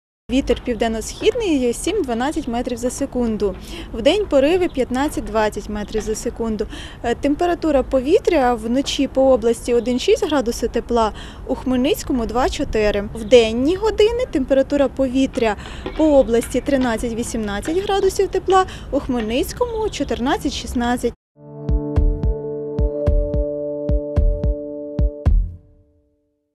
Завтра на Хмельниччині передбачається вітряна погода без опадів
Завтра на Хмельниччині передбачається вітряна погода без опадів. Вночі можливі заморозки на поверхні ґрунту від нуля до трьох градусів морозу. Про це повідомила синоптик Хмельницького обласного центру з гідрометеорології Юлія ГРАБАР.